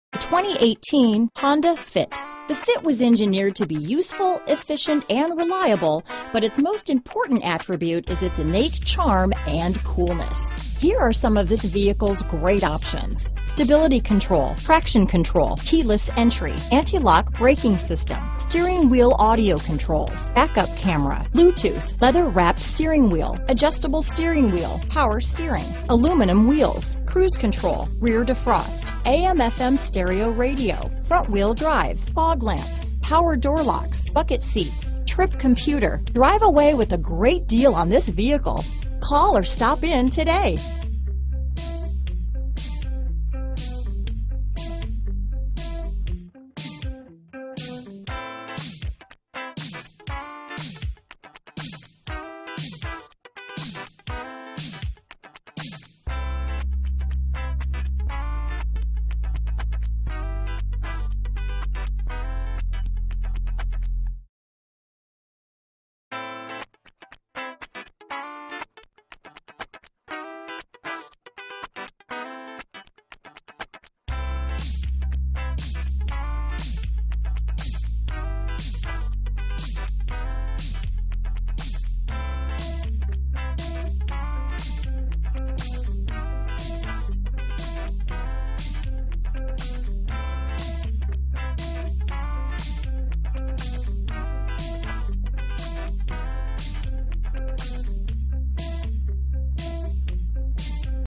2018 Honda Fit San Antonio, Austin, Houston, Boerne, Dallas, TX H180045
2018 Honda Fit

Gunn Honda
14610 IH-10 W

2018 Honda Fit Sport Small in size and big in attitude, the redesigned 2018 Fit is the right mix of modern sport styling and aerodynamic efficiency. The body lines and contours flow into the headlights, grille and badging, creating a unique front-end with attitude. Clever interior design, numerous amenities and excellent visibility for driver and passengers combine for an enjoyable, airy ride. Steering wheel-mounted controls help you keep your hands on the wheel while controlling the audio, phone, voice command and cruise control functions. The six-speed manual transmission lets enthusiasts take control of the Fit for a more sensory driving experience. Race-inspired paddle shifters on the steering wheel allow you to control your shifts for a more spirited driving experience. The unique 60/40 Split 2nd-Row Magic Seat has four clever configurations to haul the passengers or cargo you want. Get a better look at what's behind you with a Multi-Angle Rearview Camera that offers three different views: normal, top-down and wide. A whole host of safety features are available on whatever style Fit best suits your needs  keeping your ride enjoyable and safe.